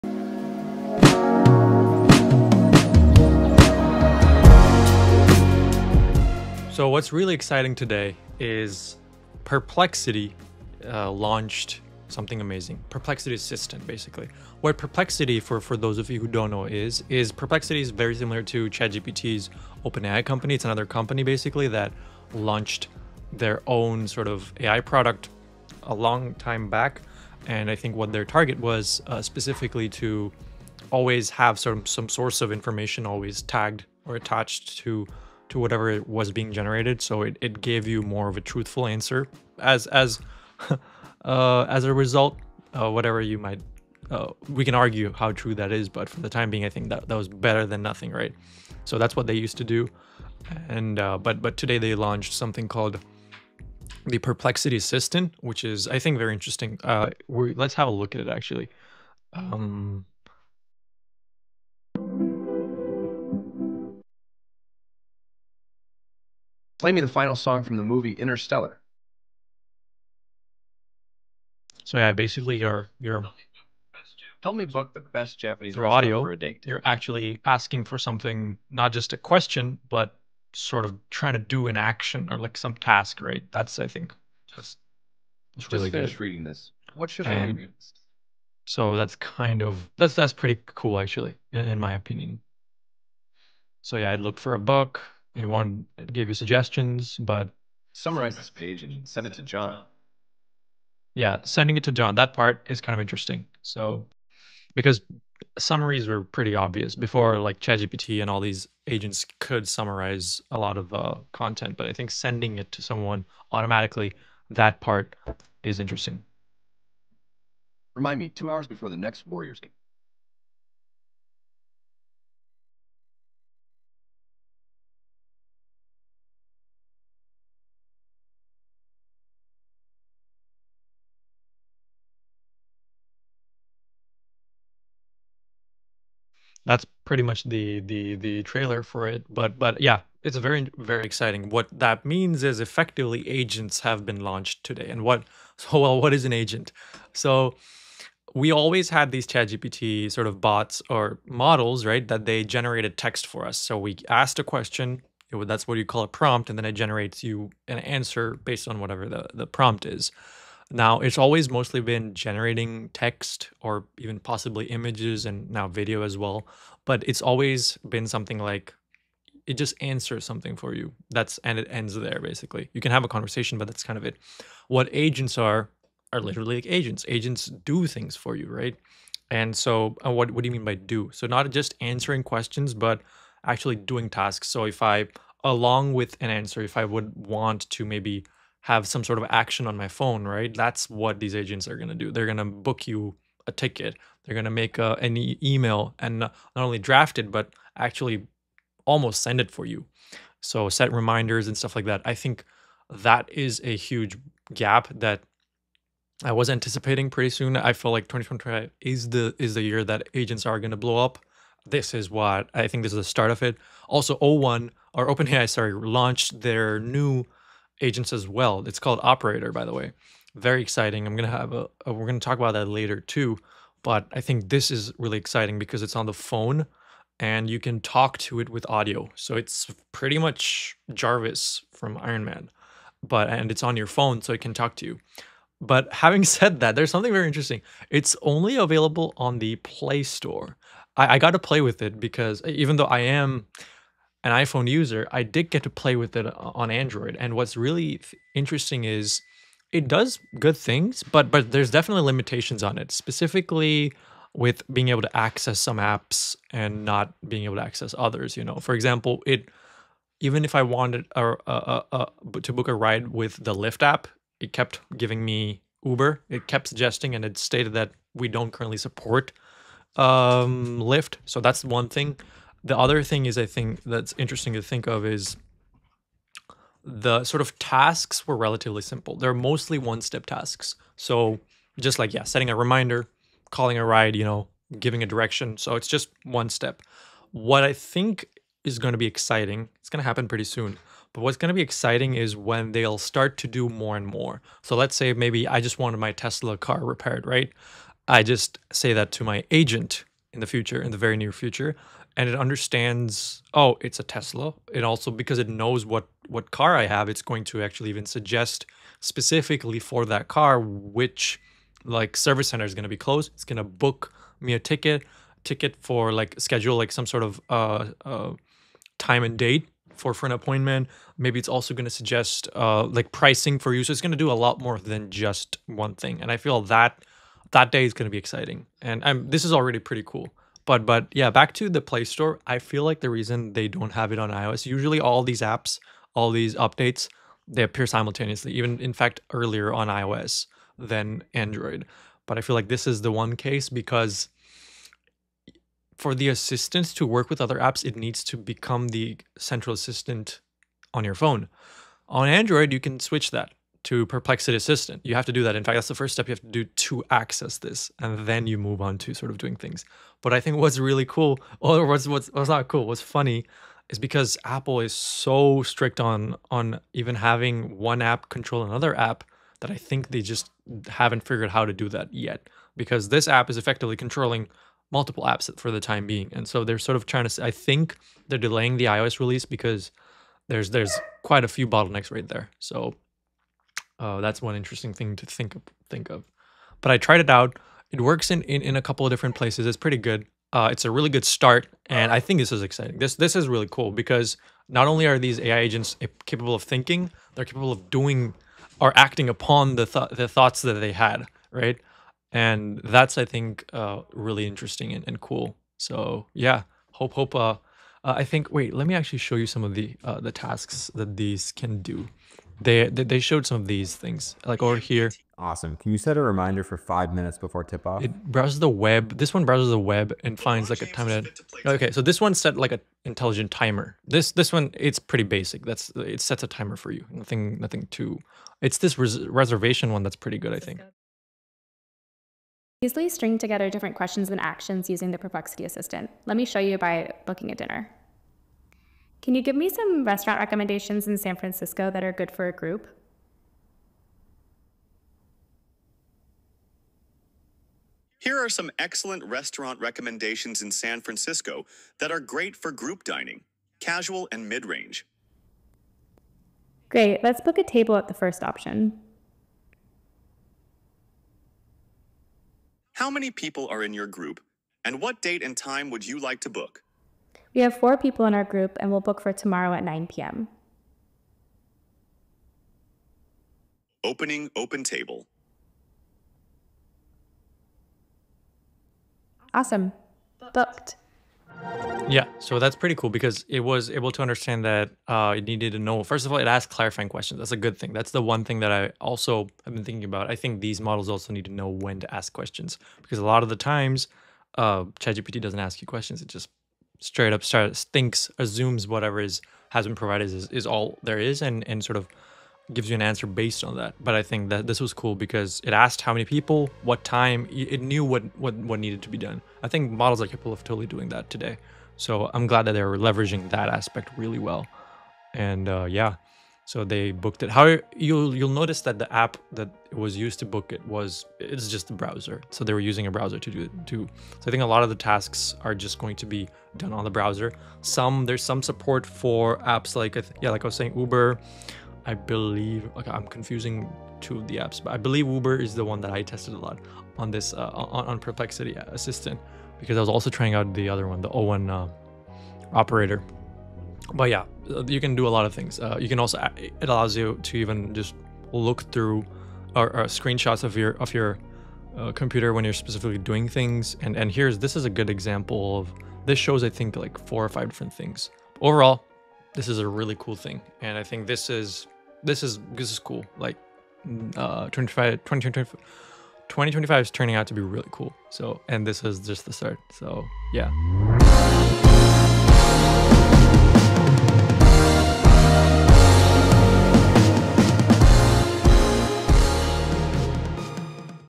2025 AI AGENTS ARE OUT!!! Perplexity just released their NEW AI ASSISTANT!!
Dive into the latest in AI technology with Perplexity's newly launched AI Assistant for Android! This video explores how this innovative tool can revolutionize your daily tasks by performing actions like booking rides, setting reminders, and more, all through voice and visual commands. Discover if this AI agent can truly transform your phone into a smarter, more efficient companion.